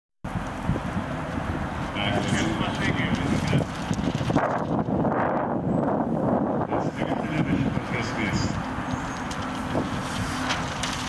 Aitvarai Vydūnui. 3
2018 kovo 21 d.
Vydūno 150 metų jubiliejaus iškilmės. Pavasario lygiadienis
Vienybės aikštė, Kaunas, Lietuva